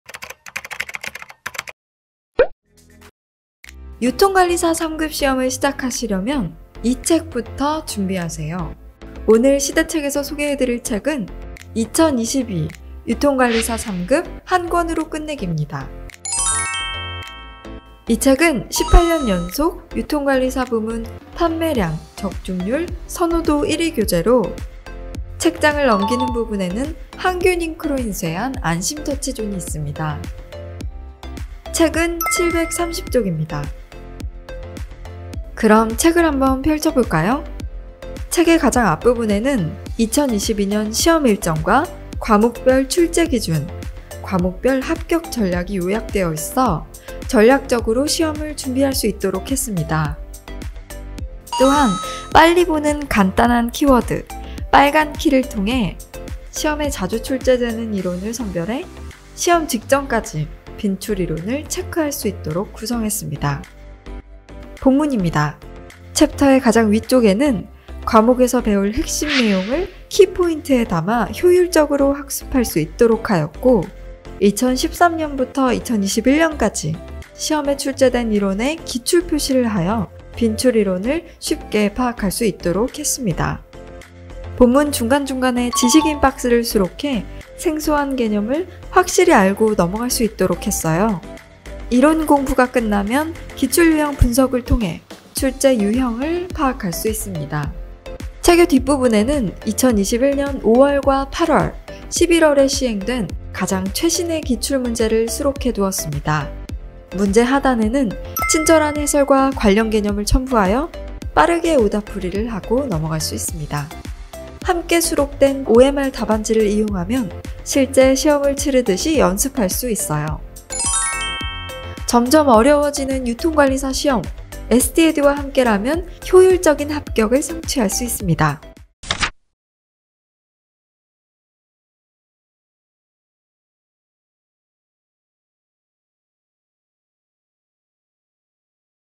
시대 책(Check)｜유통관리사 3급 이론부터 기출까지 한권으로 꽉 잡아!｜자격증,스펙,대기업,취업,인강,강의,기출문제,모의고사,교재,책
서점 갈 필요 없는, 도서체크 - 시대 책(Check)

[2022 유통관리사 3급 한권으로 끝내기]
서점에 가지 않고도 2분만에 도서 살펴보기

유통관리사 합격의 명가 SD에듀(시대에듀)!
최고의 교재와 최고의 강의로
단기간에 유통관리사 합격을 드립니다